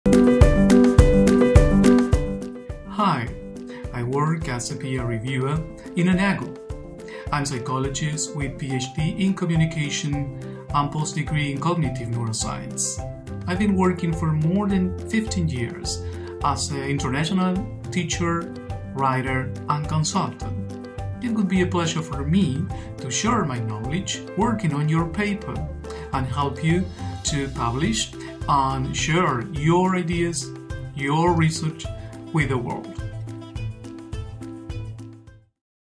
Enago Publication Expert - Most Trusted Subject Area Expert for Academic Editing
Who can better help you communicate your research to an international audience than a psychologist with a PhD in Communication who has spent more than 15 years teaching and consulting internationally? His extensive publication history includes 10+ published papers and 18+ authored/ co-authored books. Meet one of our publication experts and learn what he could help you with!